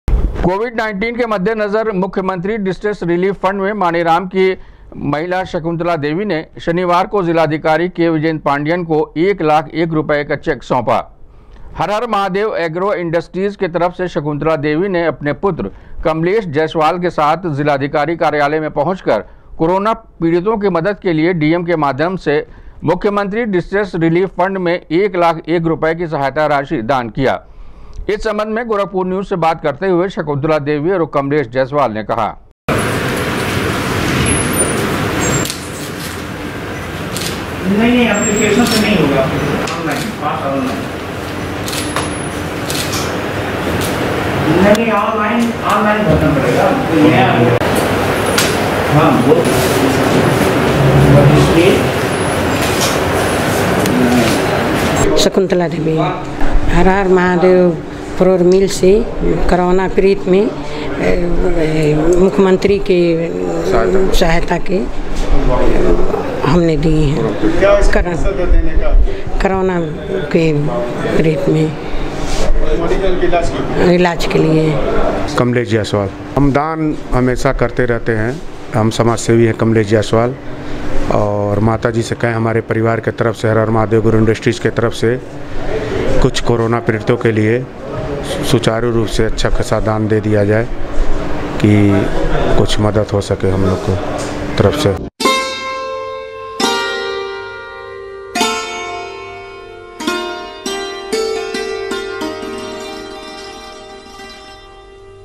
कोविड19 के मद्देनजर मुख्यमन्त्री डिस्ट्रेस रिलीफ फण्ड में हर हर महादेव इंडस्ट्रीज ने डीएम के
कोविड19 के मद्देनजर मुख्यमन्त्री डिस्ट्रेस रिलीफ फण्ड में हर हर महादेव इंडस्ट्रीज ने डीएम के माध्यम से दिया एक लाख एक रुपये की सहायता ,कहा कोरोना पीड़ितों के लिए है प्रयास।।